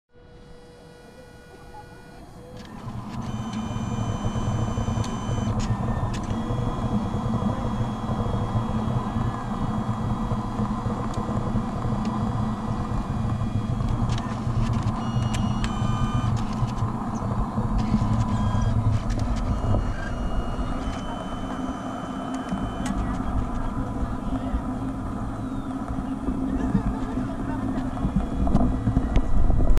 Keluarga AJ di Bandung - Kawah Putih, Bandung
Kawah Putih is a striking crater lake and tourist spot in a volcanic crater about 50 km south of Bandung in West Java in Indonesia. Kawah Putih lake is one of the two craters which make up Mount Patuha, an andesitic stratovolcano. Mount Patuha is one of numerous volcanoes in Java.